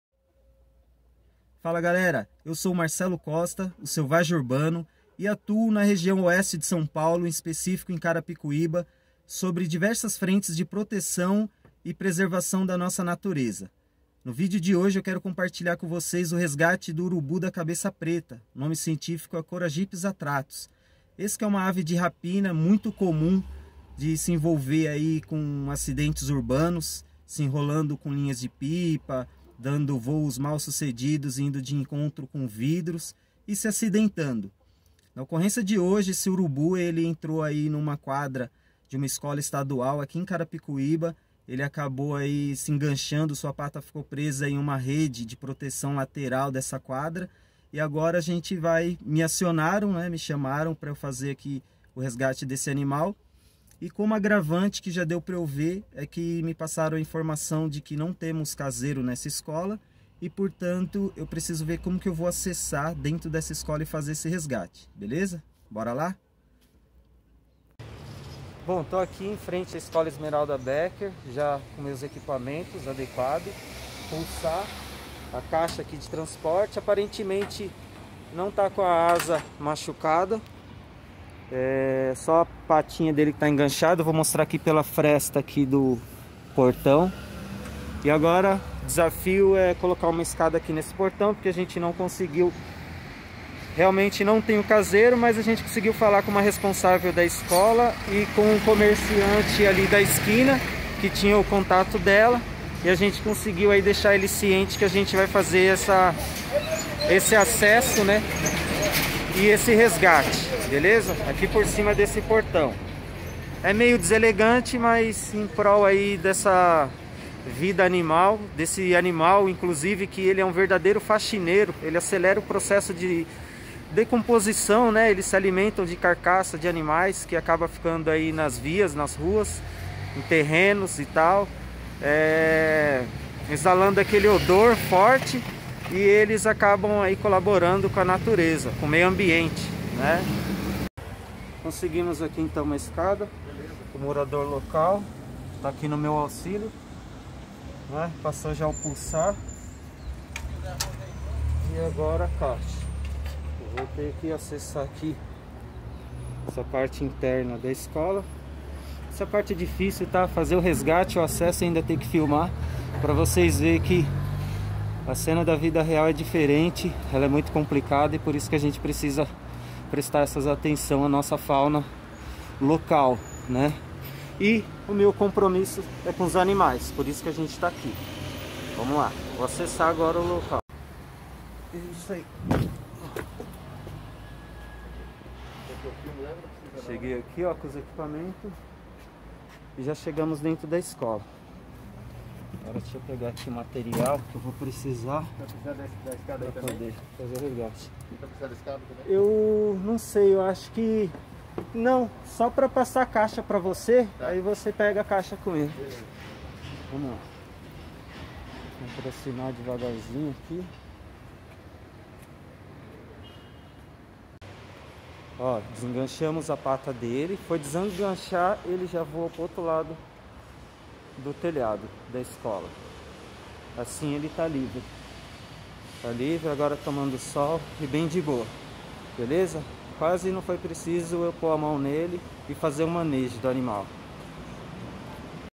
Mais um acidente envolvendo animais silvestres
Um urubu ( coragyps atratus) estava com uma pata enrolada numa tela de quadra escolar, estiquei a tela de um lado pro outro e liberei o animal, a melhor parte não consegui filmar.